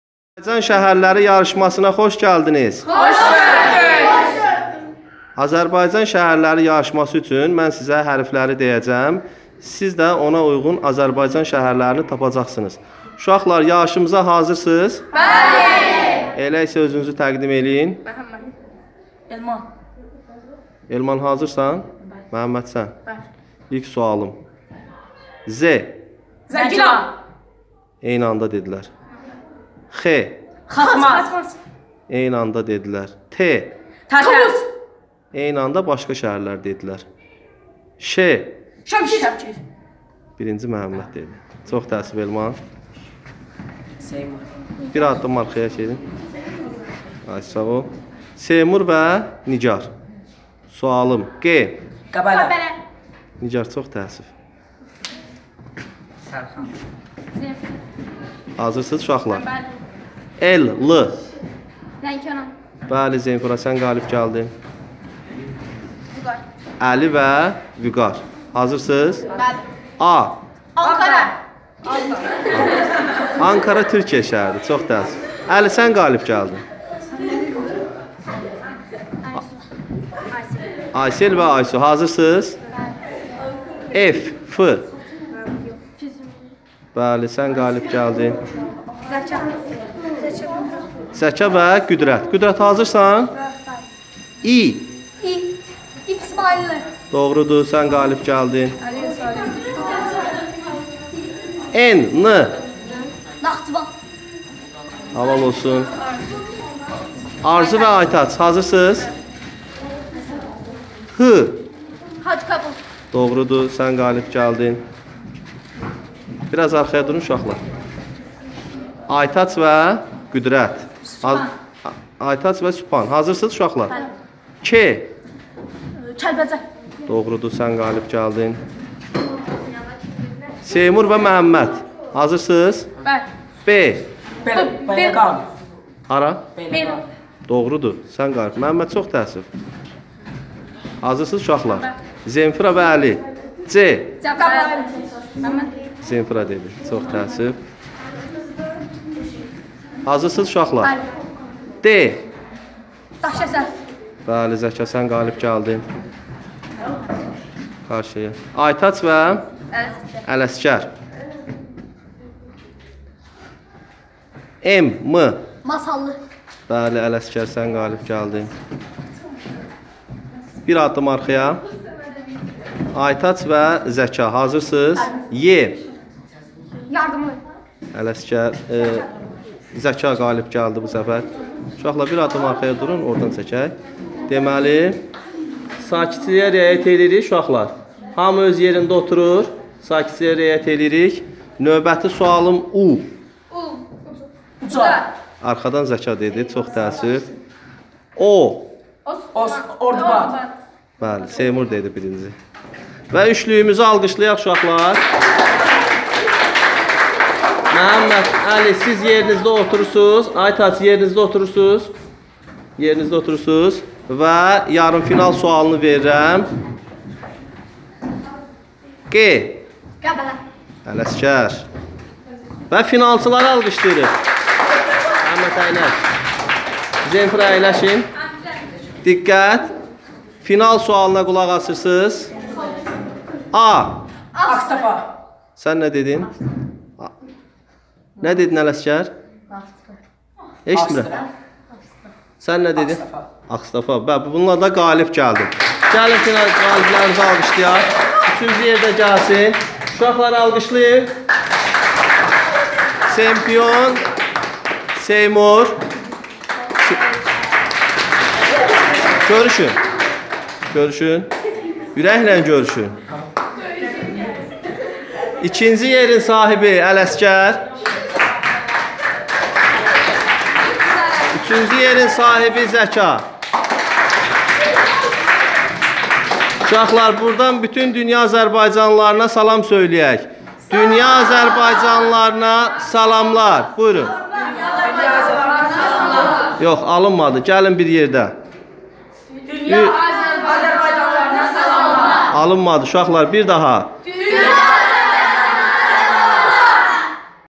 Azərbaycan Şəhərləri Yarışması -2 (Sürpriz Anlar)
GƏLİN SƏYAHƏTƏ ÇIXAQ YARIŞMAMIZI DAVAM ETDİRƏRƏK İKİNCİ DƏFƏ AZƏRBAYCAN ŞƏHƏRLƏRİ ÜZRƏ YARIŞDIQ...
Azərbaycan, Gəncə şəhəri 41 saylı tam orta məktəb. Arzu müəllim

ABUNƏ OLMAĞI, LİKE ATIB, RƏY YAZMAĞI UNUTMAYIN.
ƏYLƏNCƏNİ PAYLAŞMAQ MƏQSƏDİLƏ BU VİDEONU PAYLAŞA BİLƏRSİNİZ...Xoş Seyrlər